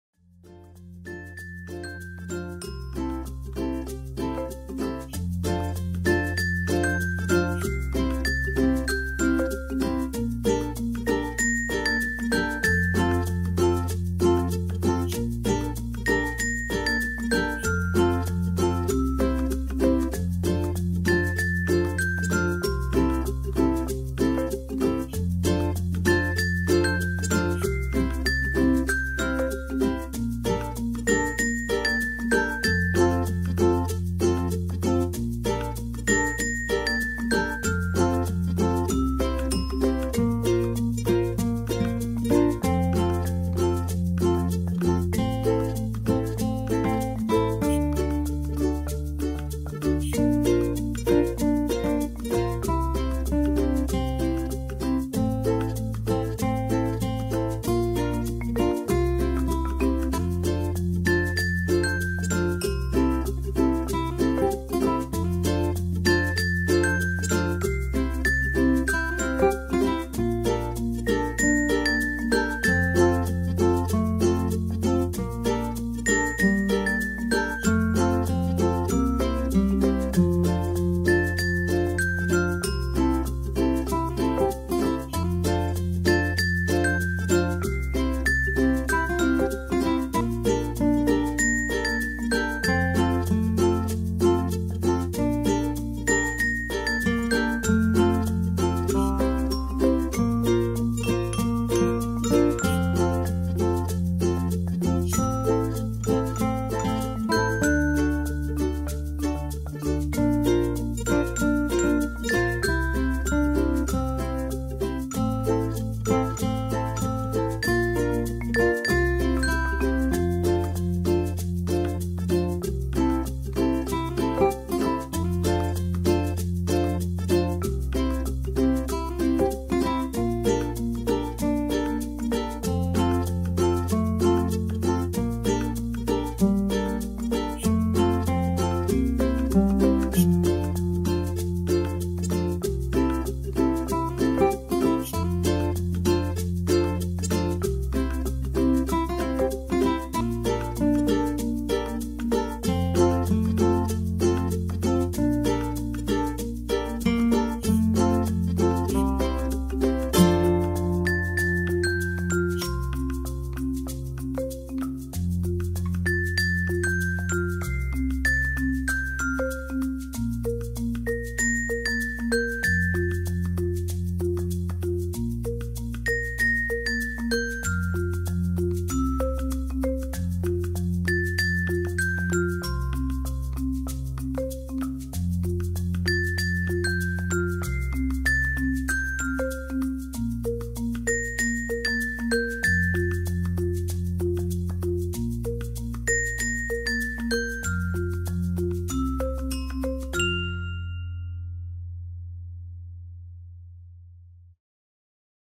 3 Bedroom House for sale in North West | Hartbeespoort Dam | Ifafi | 82 Boem Crescent
3 Bedroom House in Ifafi, 82 boem crescent, There is something about this home that ensures you can wind down as you enter the door. This home o